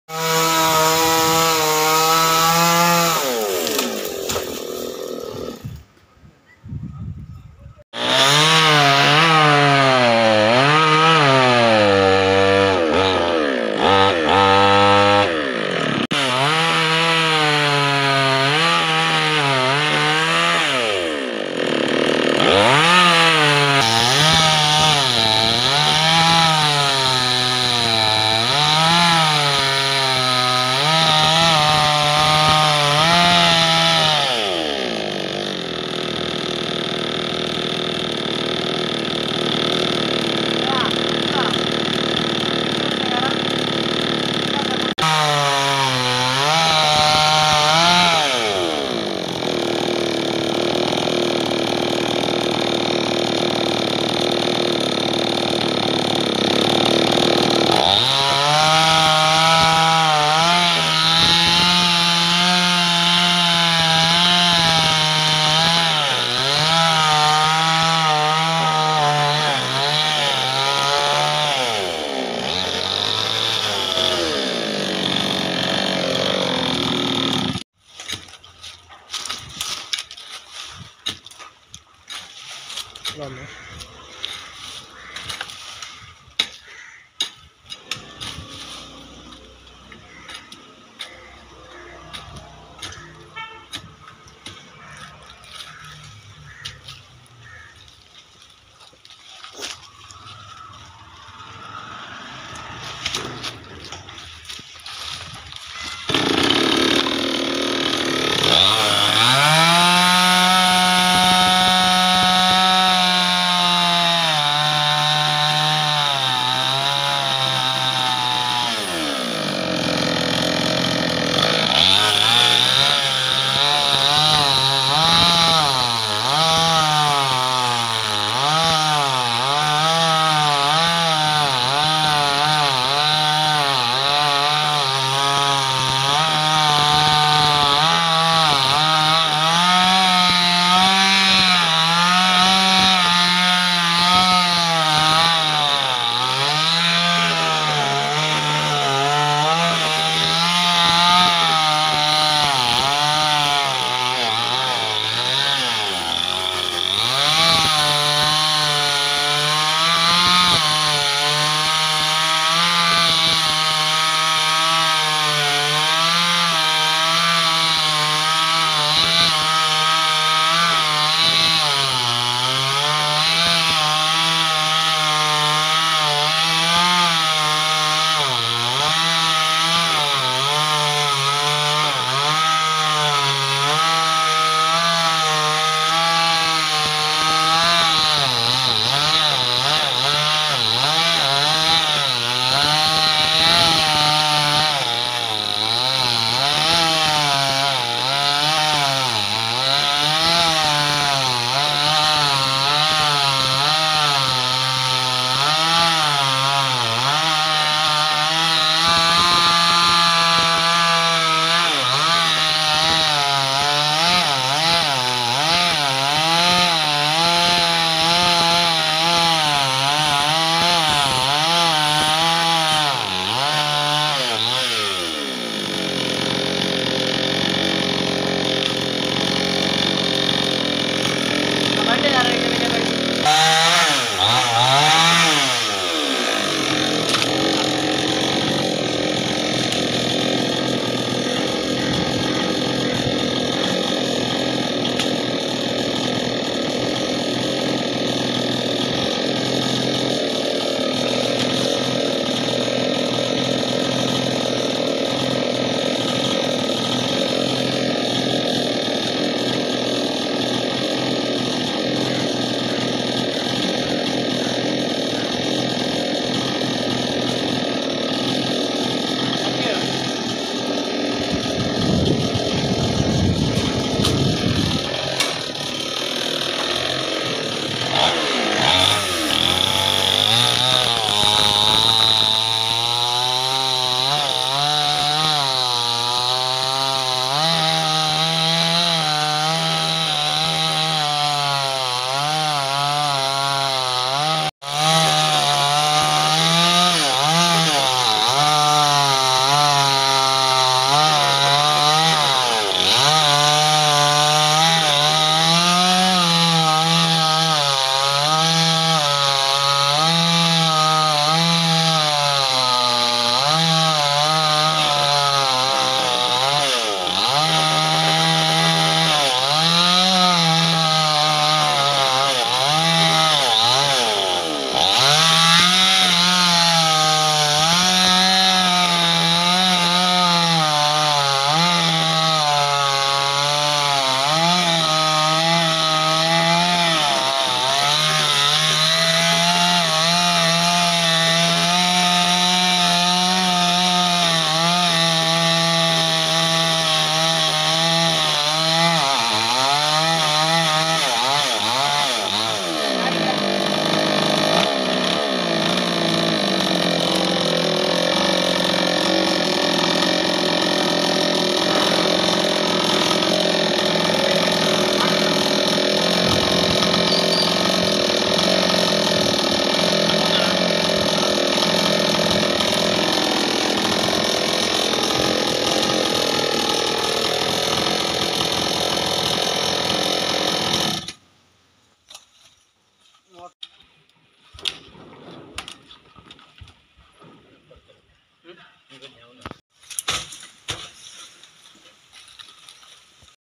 Mango Tree Cutting With Petrol Chainsaw Machine | Big Mango Tree Cutting
Today my video big mango tree cutting with chainsaw machine.